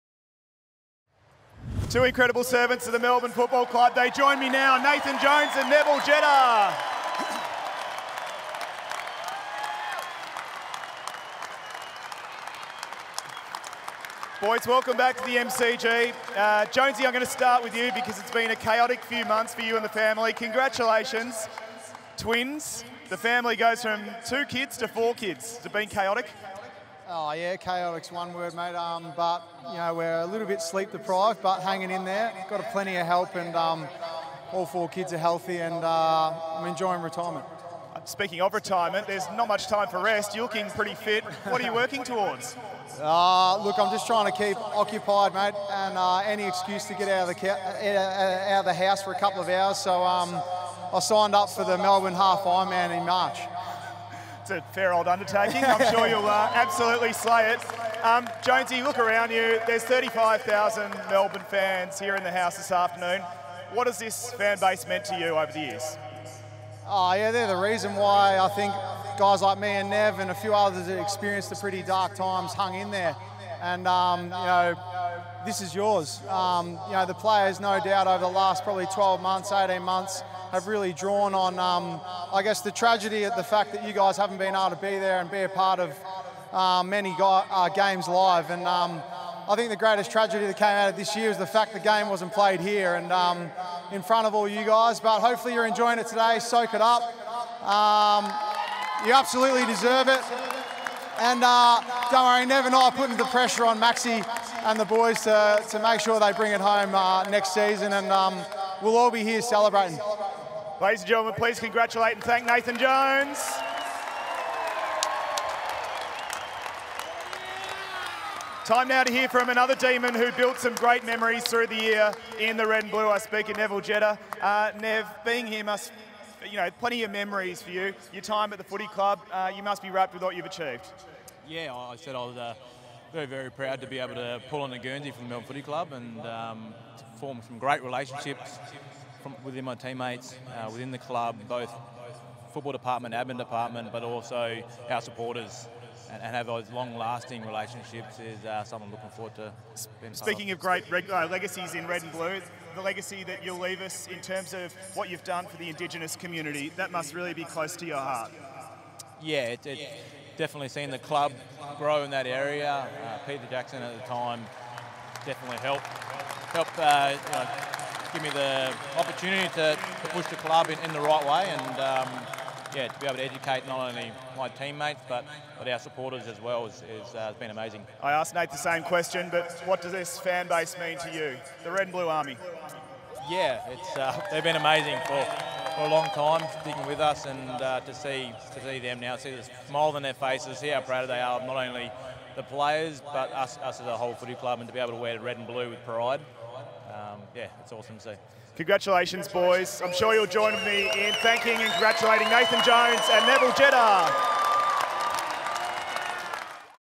Premiership Celebration | Nathan Jones & Neville Jetta
The club pays tribute to retiring superstars Nathan Jones & Neville Jetta